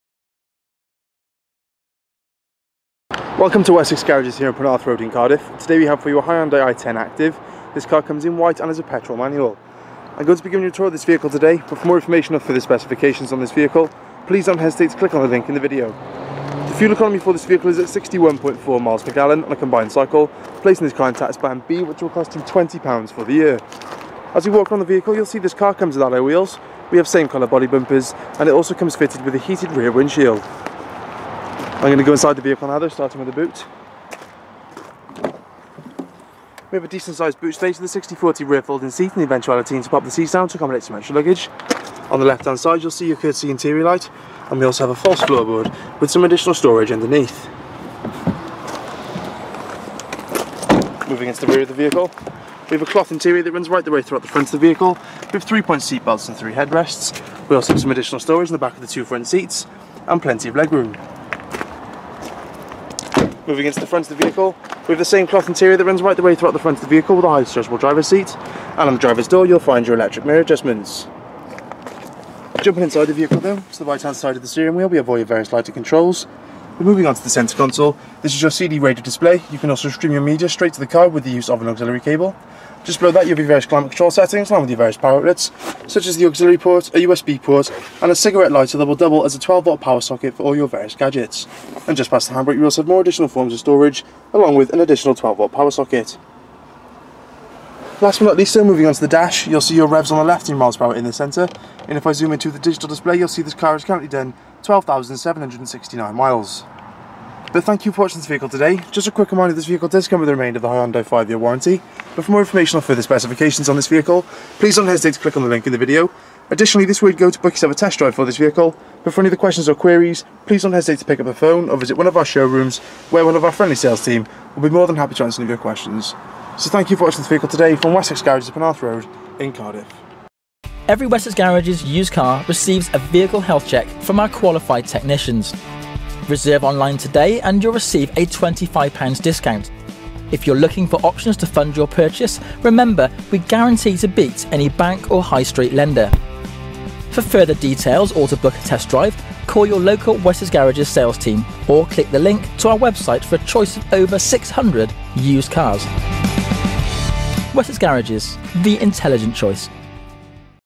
Wessex Garages Penarth Road Cardiff, Used, Hyundai i10 ACTIVE, PETROL, MANUAL, CN63XSM
This vehicle was first registered in 2013 and has 1 previous owners.